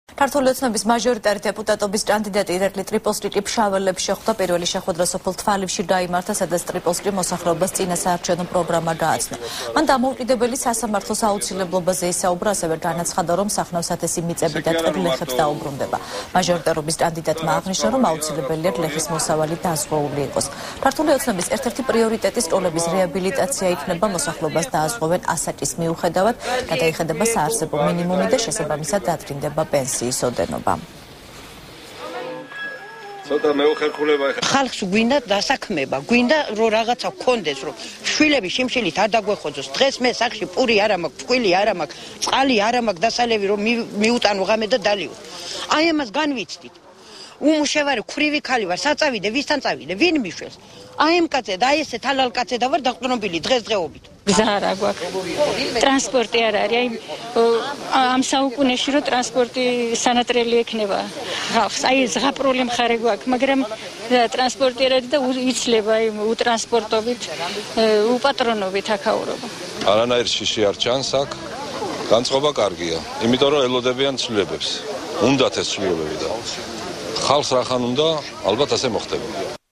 ახალი 3 | შეხვედრა მოსახლეობასთან | 18.08.12
"ქართული ოცნების" მაჟორიტარი დეპუტატობის კანდიდატი ირაკლი ტრიპოლსკი ფშაველებს შეხვდა. პირველი შეხვედრა სოფელ თვალივში გაიმართა, სადაც ტრიპოლსკიმ მოსახლეობას წინასაარჩევნო პროგრამა გააცნო. მან დამოუკიდებელი სასამართლოს აუცილებლობაზე ისაუბრა, ასევე განაცხადა რომ სახნავ-სათესი მიწები და ტყე გლეხებს დაუბრუნდება. მაჟორიტარობის კანდიდატმა აღნიშნა, რომ აუცილებელია გლეხის მოსავალი დაზღვეული იყოს. "ქართული ოცნების" ერთ-ერთი პრიორიტეტი სკოლების რეაბილიტაცია იქნება. მოსახლოებას დააზღვევენ ასაკის მიუხედავად, გადაიხედება საარსებო მინიმუმ და ესაბამისად დადგინდება პენსიის ოდენობა.